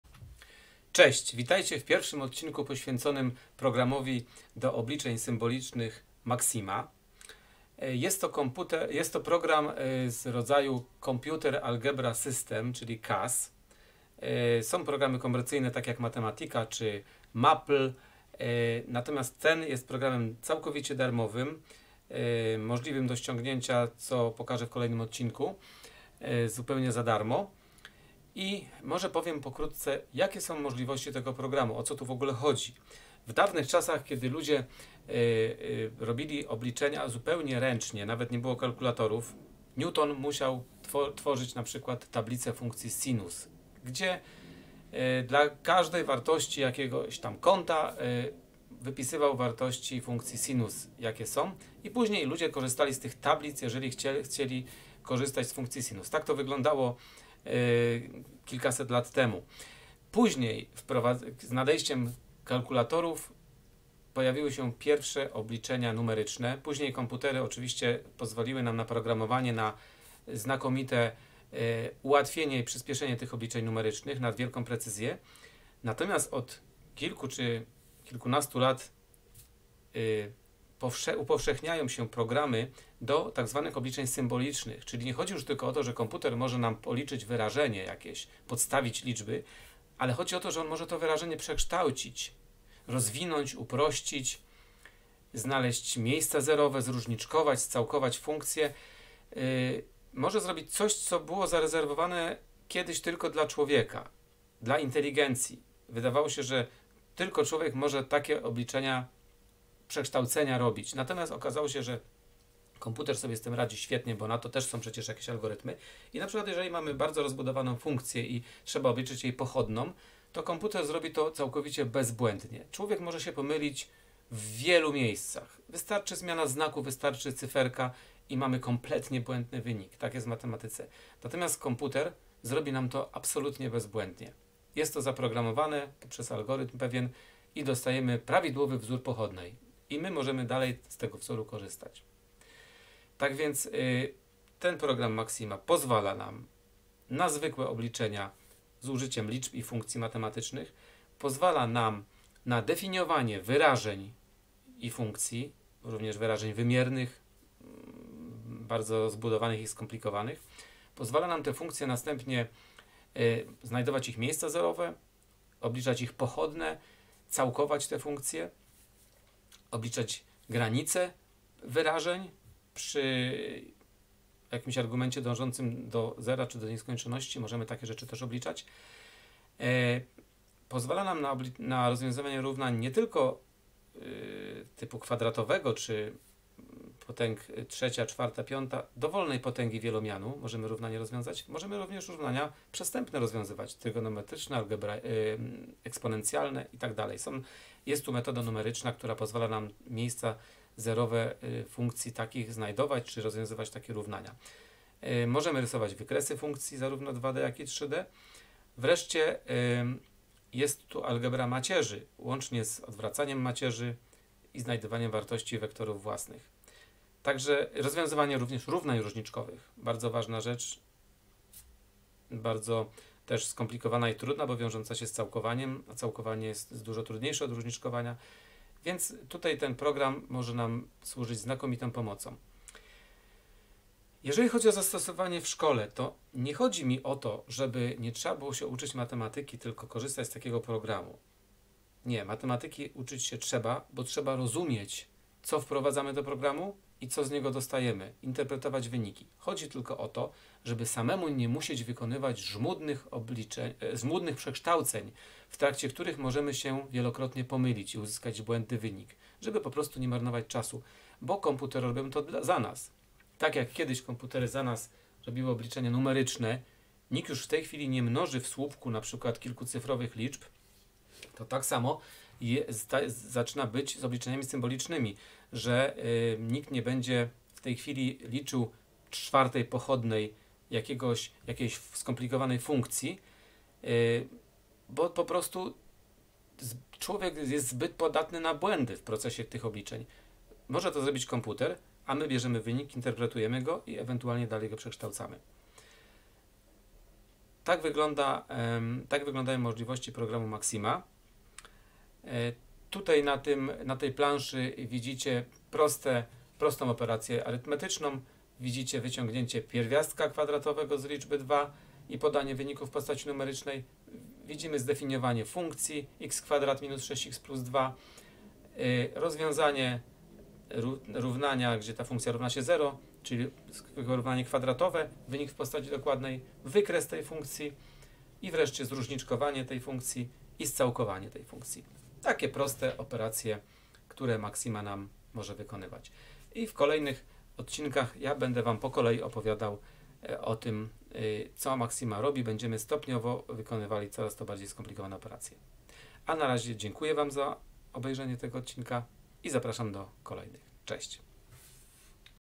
Maxima odc. 1 - ogólnie o programie Maxima
Garść informacji czym jest w ogóle program Maxima